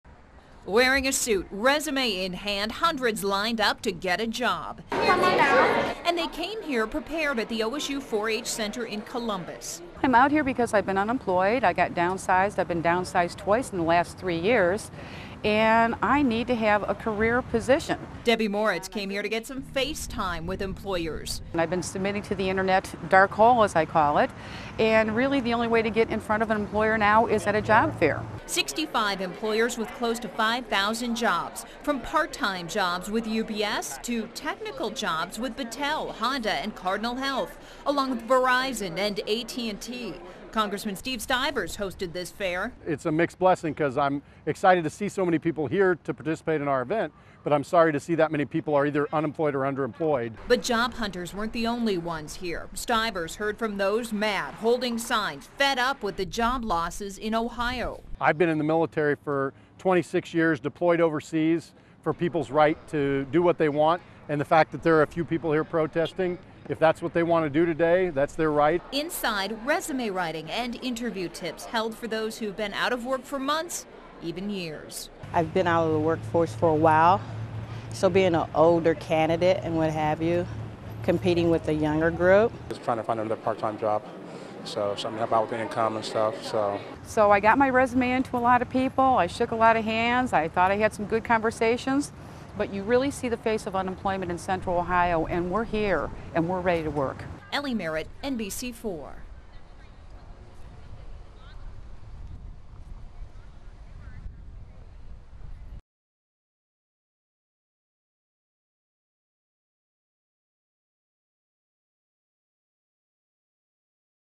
Stivers' Job Fair
Congressman Steve Stivers hosted a job fair on the OSU campus today. NBC4 talks to jobseekers and Stivers.